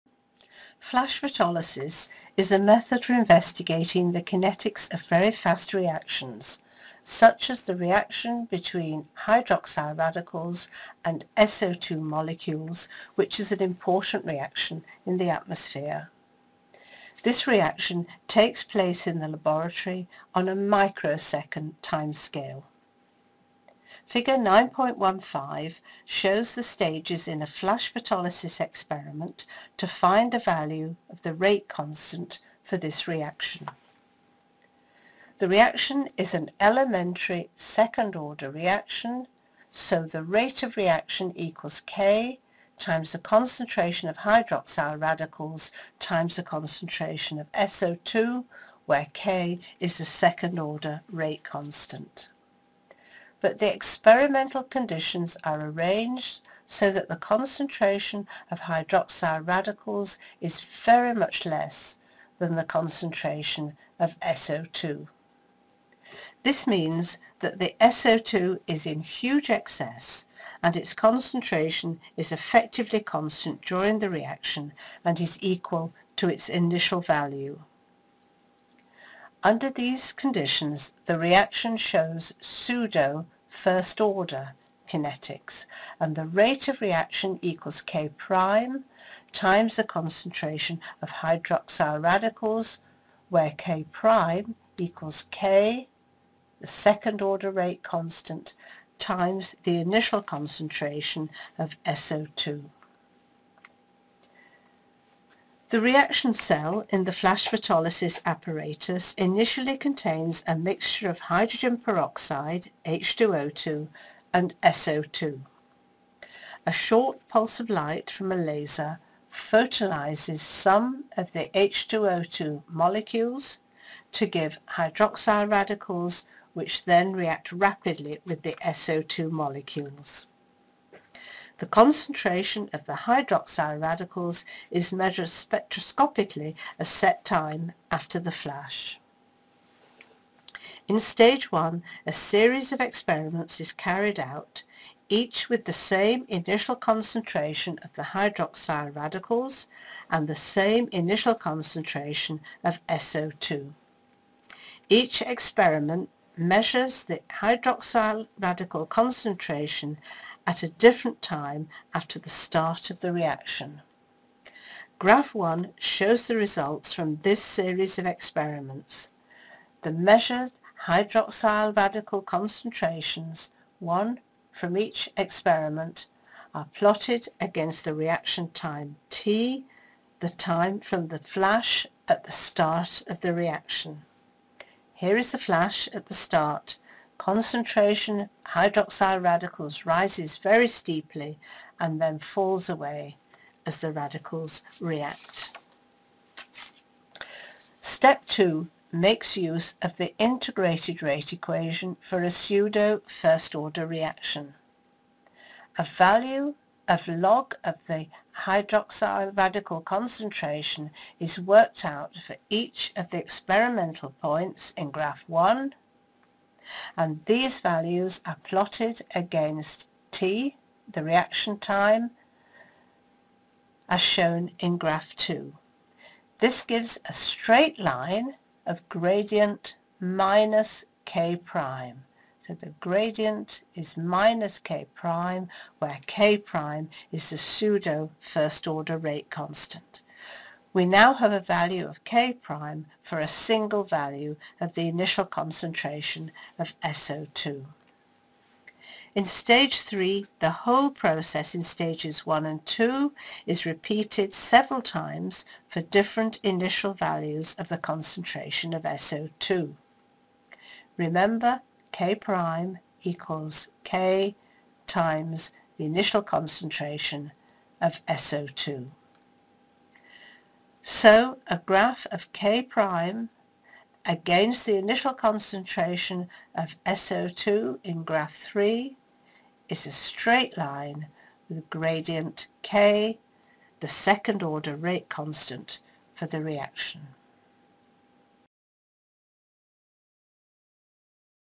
Using flash photolysis to find the rate constant
In this screencast, Gwen Pilling walks you through Figure 9.15 to explain the stages in a flash photolysis experiment to find the rate constant for the reaction of •OH radicals with SO2.